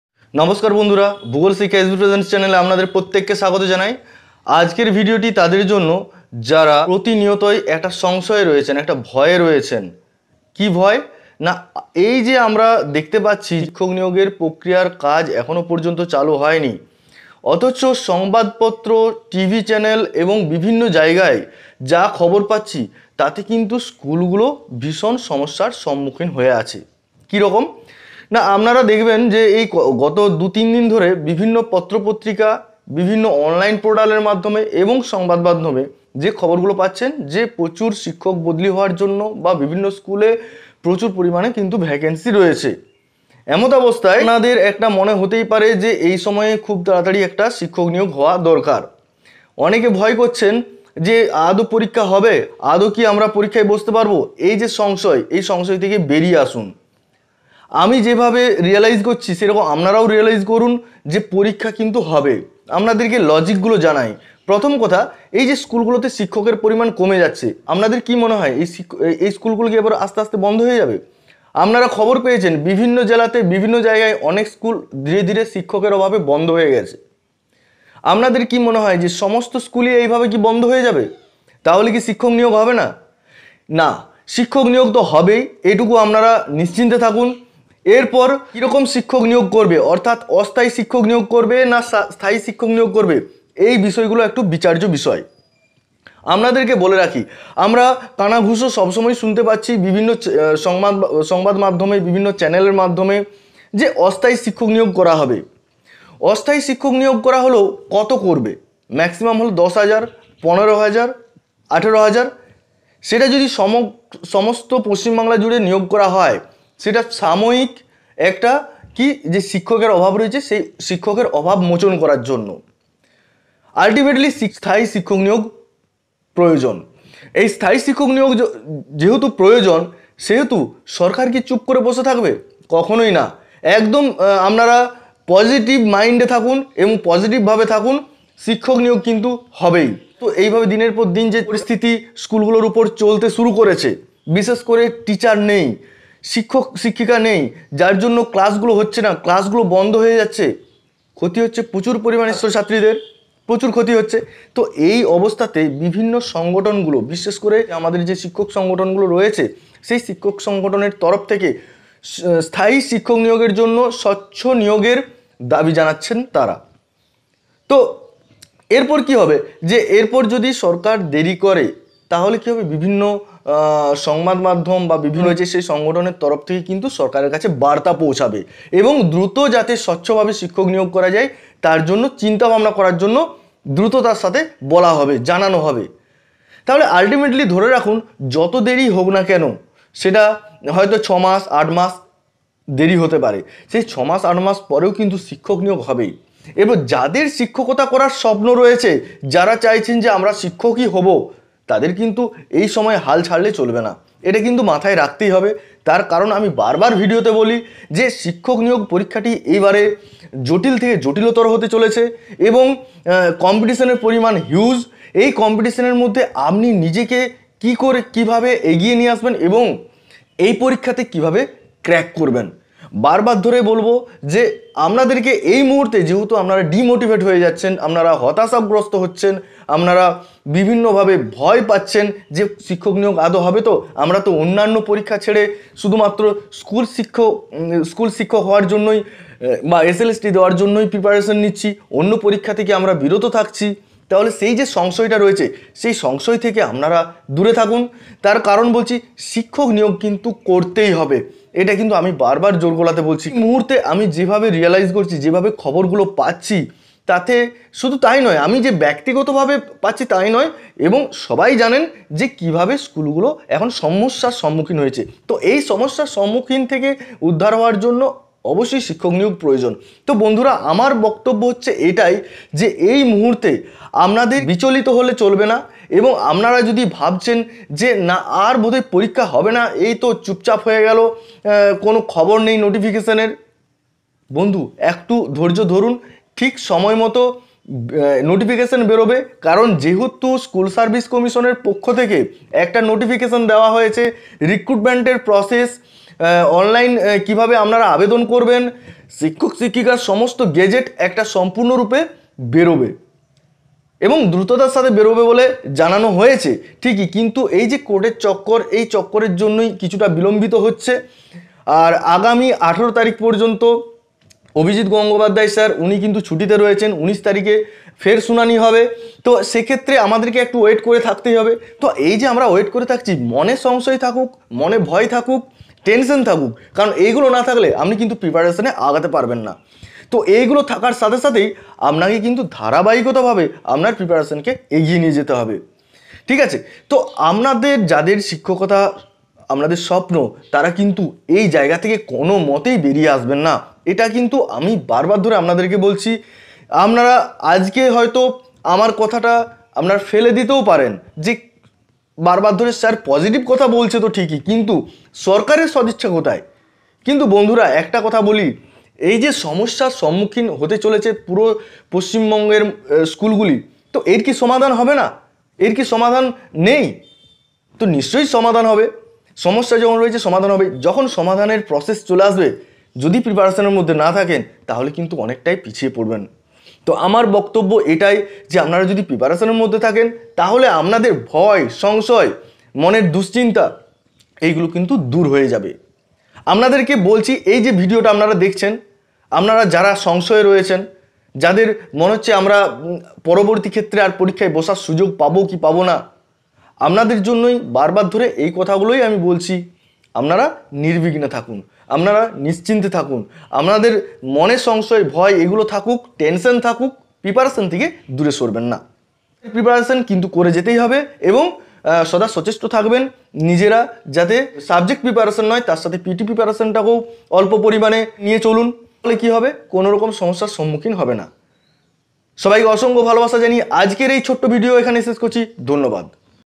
SLST নিয়ে দুশ্চিন্তা? এখন কি করণীয়?
আজকের আলোচ্য বিষয় slst নিয়ে দুশ্চিন্তা, হতাশা, সংশয়?